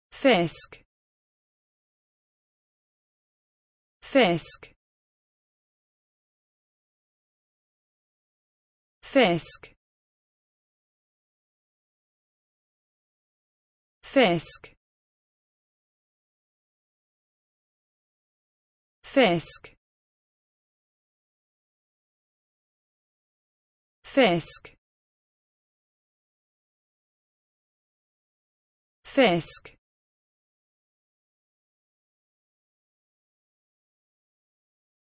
the correct pronunciation of fisk in English.
This video shows you how to pronunciation fisk in English.